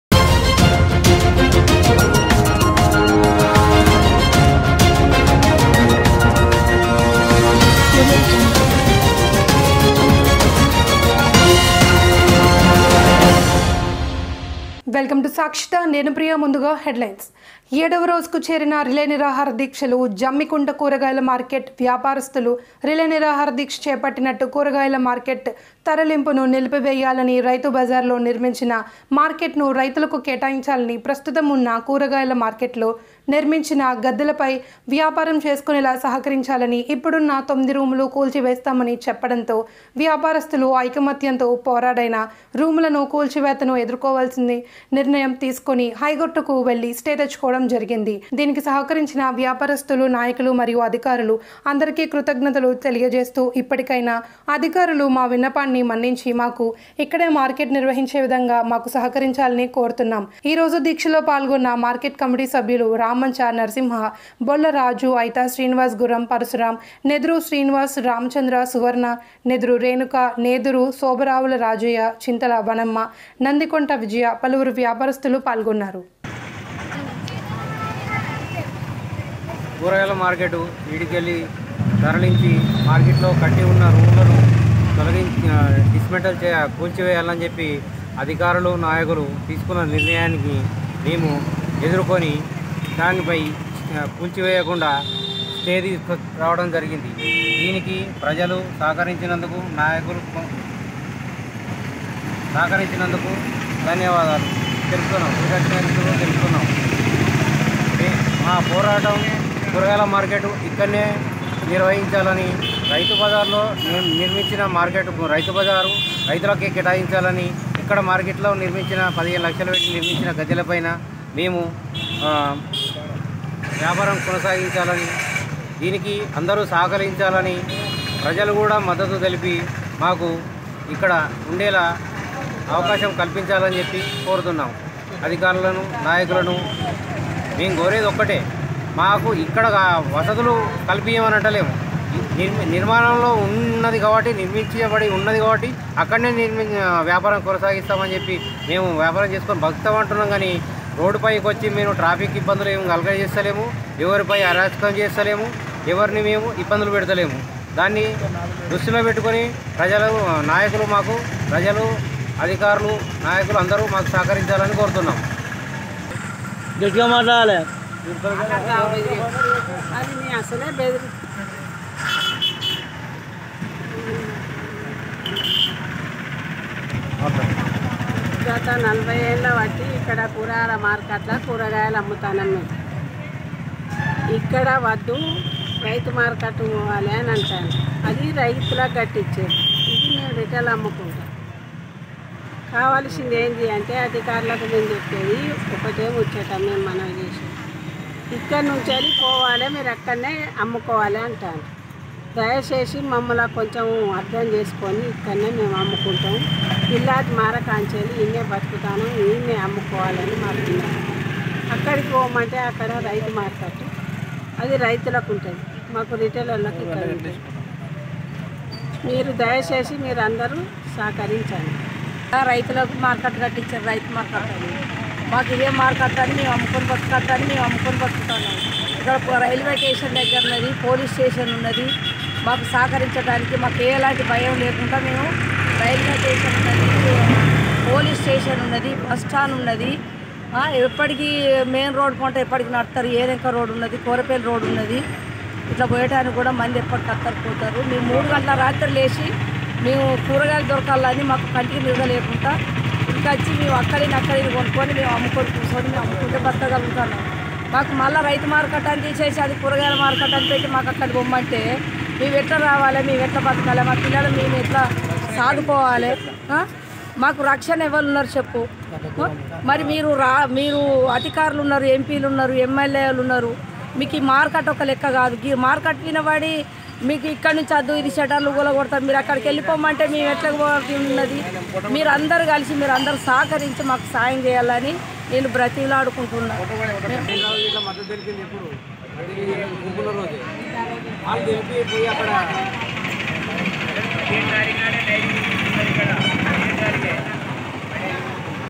7వ రోజుకు చేరిన రిలే నిరాహారా దీక్షలు | SAKSHITHA NEWS
7వ రోజుకు చేరిన రిలే నిరాహారా దీక్షలు       జమ్మికుంట కూరగాయల మార్కెట్ వ్యాపారస్తులు రిలే నిరాహార దీక్ష చేపట్టి కూరగాయాల మార్కెట్ తరలింపును నిలిపివేయాలని రైతు బజార్ లో నిర్మించిన మార్కెట్ ను రైతులకు కేటాయించాలని ప్రస్తుతం ఉన్న కూరగాయాల మార్కెట్ లో నిర్మించిన గద్దెల పైన వ్యాపారం చేసుకునేలా సహకరించాలని ఇప్పుడున్న 9 రూములు కూల్చివేస్తామని చెప్పడంతో వ్యాపారస్తులు ఐక్యమత్యంతో పోరాడైన రూములును కూల్చివేతను ఎదుర్కోవాలని నిర్ణయం తీసుకొని హైకోర్టుకు వెళ్లి స్టే తెచ్చుకోవడం జరిగింది దీనికి సహకరించిన వ్యాపారస్తులు  నాయకులు మరియు అధికారులు అందరికీ కృతజ్ఞతలు తెలియజేస్తు ఇప్పటికైనా అధికారులు మా విన్నపాన్ని మన్నించి మాకు ఇక్కడే మార్కెట్ నిర్వహించే విధంగా మాకు సహకరించాలని కోరుతున్నాం ఈరోజు దీక్షలో పాల్గొన్న మార్కెట్ కమిటీ సభ్యులు రామంచ నరసింహ బొల్ల రాజు అయిత శ్రీనివాస్ గుర్రం పరశురాం నేదురు శ్రీనివాస్ రామంచ సువర్ణ నేదురు రేణుక నేదురు శోభ రావుల రాజయ్య చింతల వనమ్మ నందికొండ విజయ పలువురు వ్యాపారస్తులు పాల్గొన్నారు